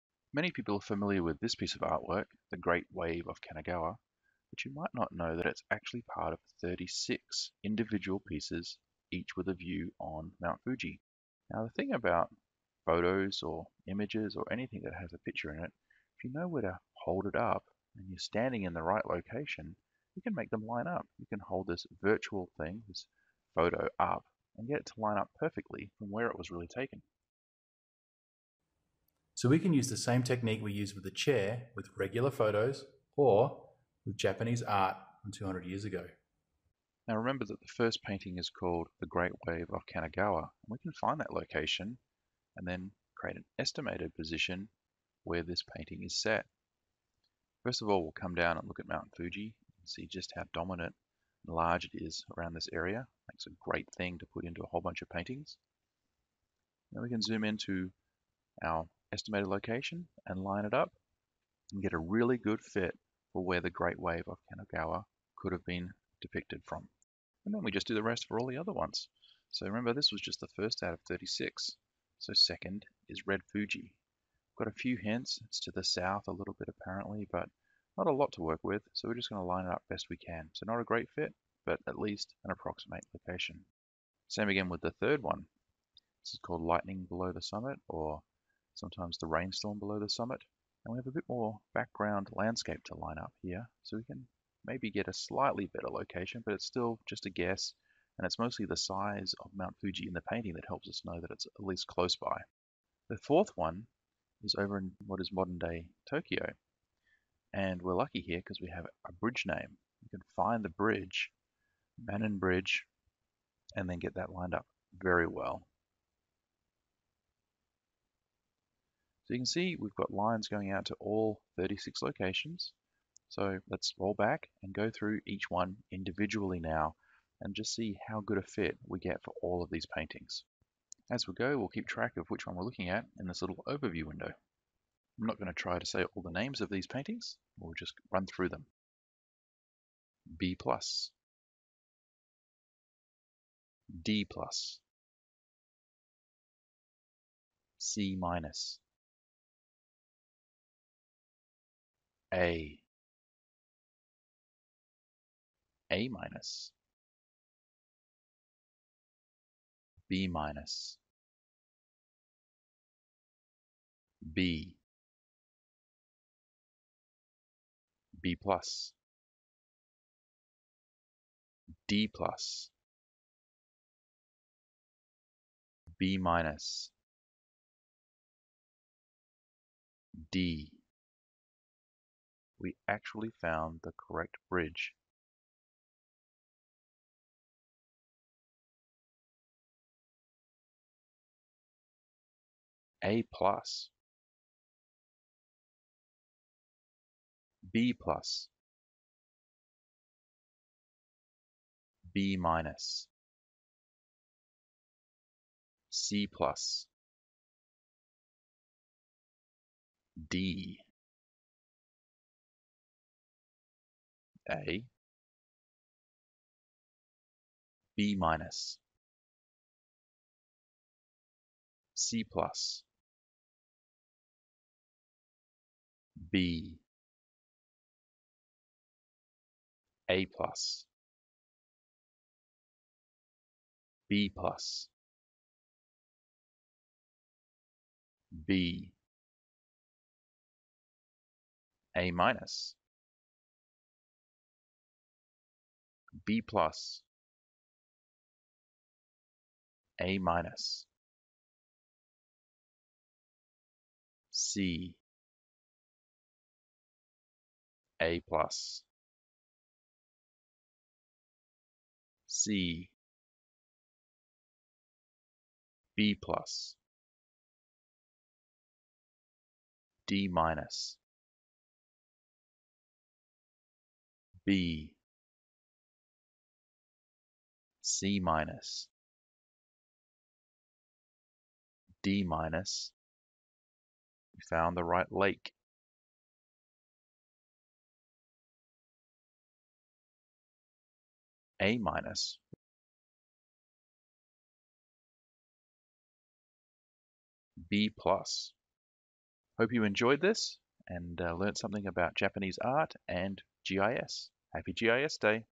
36 (Virtual) Views of Mt Fuji
Hokusai's "36 Views of Mt Fuji" are beautiful Japanese works of art from the early 1800's that all contain a view of Mt Fuji, situated to the west of Tokyo.

The placement and size of Mt Fuji in the painting, together with its name and other information, allows us to estimate where the artist was (or imagined he was) when creating the artwork 200 years ago. In a few cases, we can estimate within a few hundred feet of this spot! To help with Q&A, each painting received a 'grade', using the classic "A+" to "D-" monikers.

This video - made for GIS Day 2015 - will show your the results!